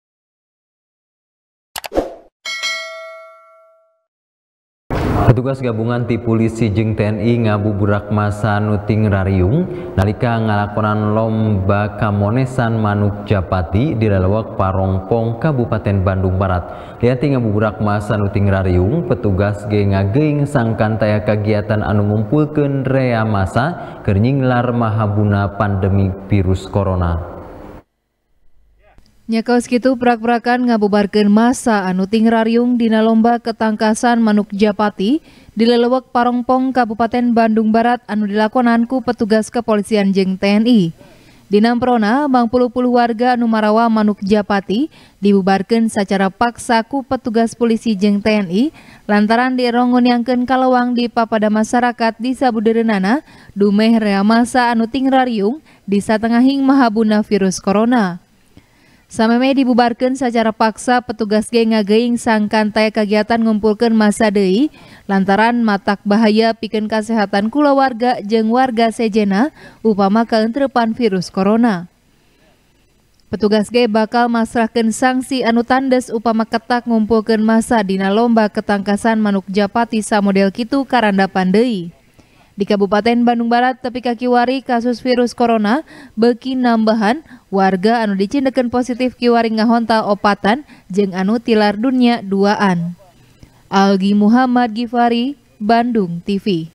Petugas Ngabubarkeun Lomba Ketangkasan Manuk Japati | TP | BANDUNG TV
TP | BANDUNG TV | IWAN M RIDWAN | Petugas Gabungan Ti Pulisi Jeung TNI, Ngabubarkeun Massa Anu Tingrariung Nalika Ngalakonan Lomba Ketangkasan Manuk Japati, Di Lelewek Parompong, Kabupaten Bandung Barat. Lian Ti Ngabubarkeun Massa Anu Tingrariung, Petugas Ge Ngageuing Sangkan Taya Kagiatan Anu Ngumpulkeun Rea Massa, Keur Nyinglar Mahabuna Pandemi Virus Corona.